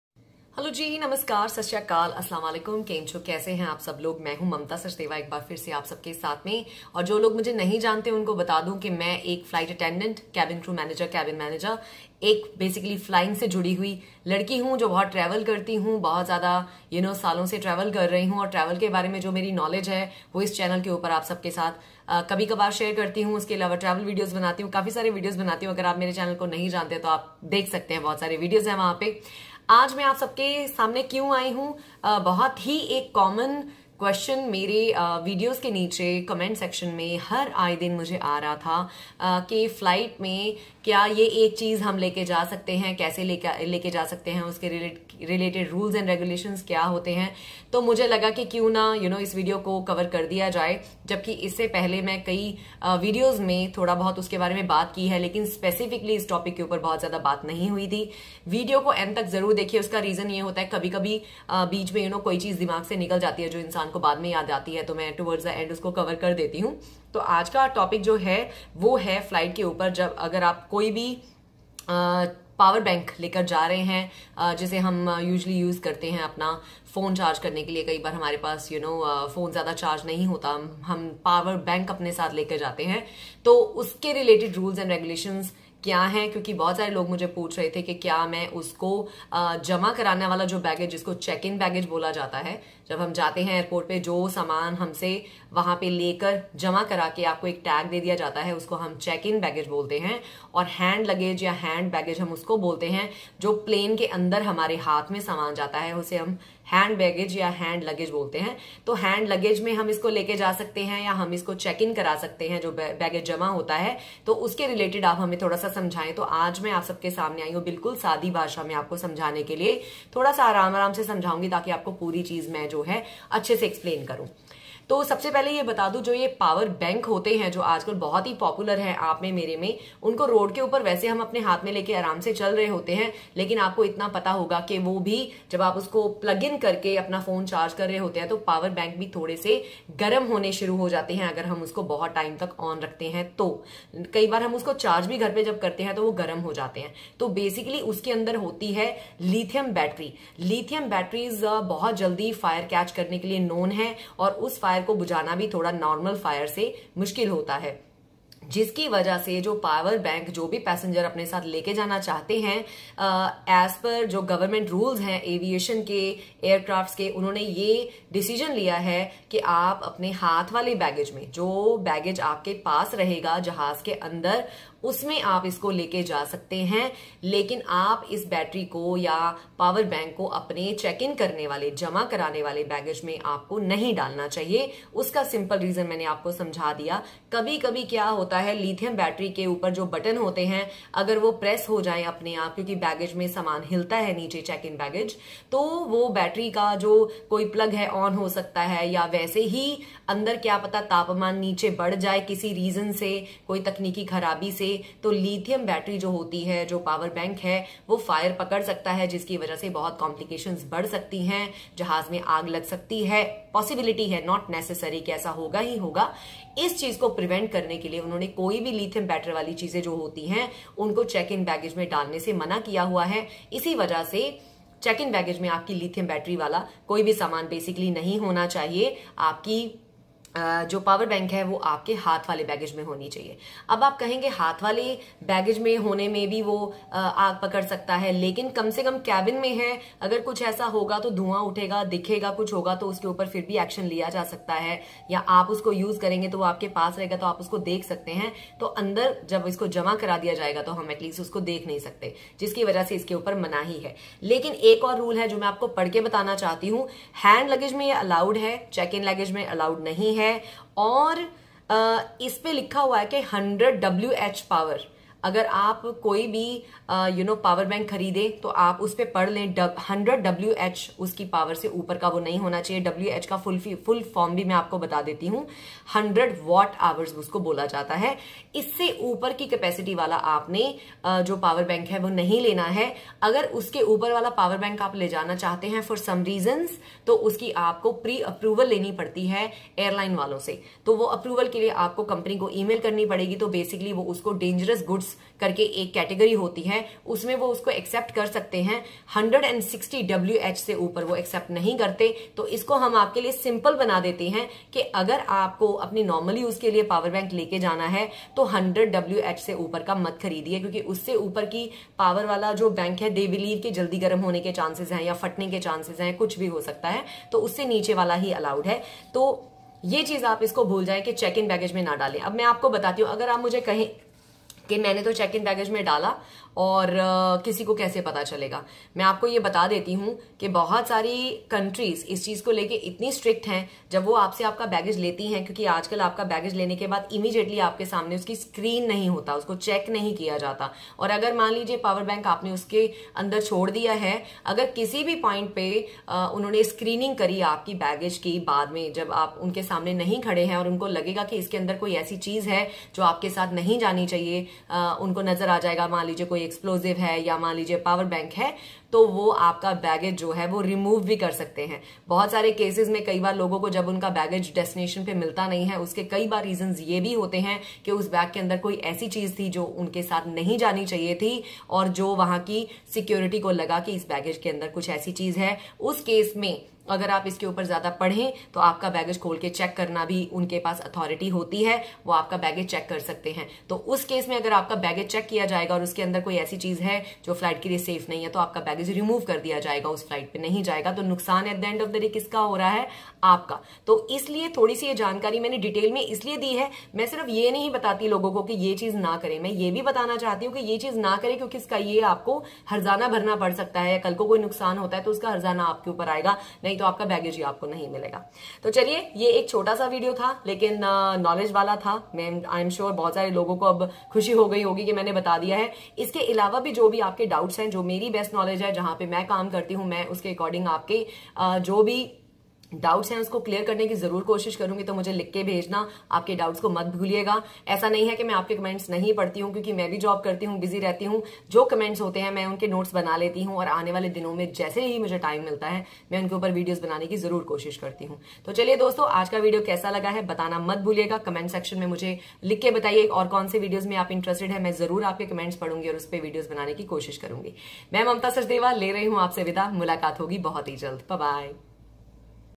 Power Bank Plane Rules, Airplane Power Bank Rules -Mamta Sachdeva
Power bank must only be carried in hand luggage or carried around. It is not allowed to carry power banks in checked luggage. If the rated power is less than 100Wh, power banks can be carried without approval; power banks with the power between 100Wh and 160Wh can be carried after approval of air carrier.